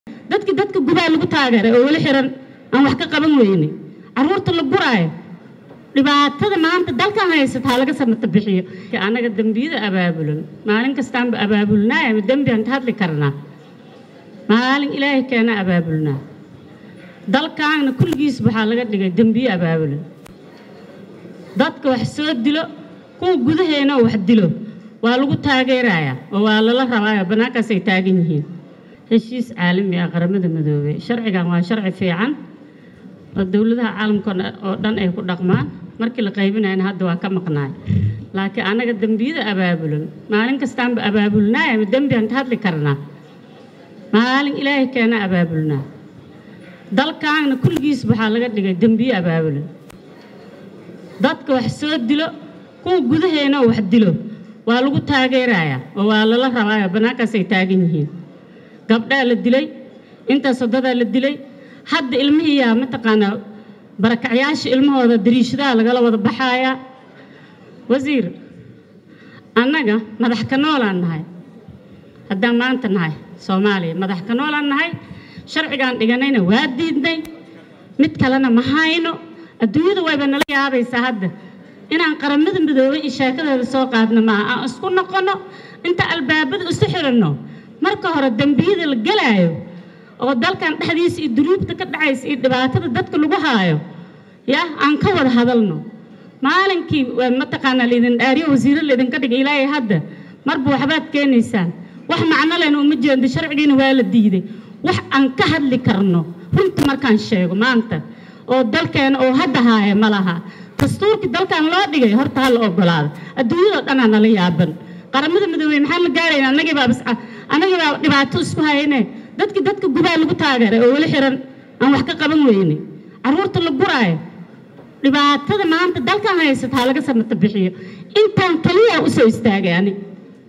"Annaga maa dambi ka hadli karna, dalkaan kuligiis waxa laga dhigay dambiyo abaabulan"xil Sareedo...
"Annaga maa dambi ka hadli karna, dalkaan kuligiis waxa laga dhigay dambiyo abaabulan"
xildhibaan Sareedo Maxamed Cabdalle, BFS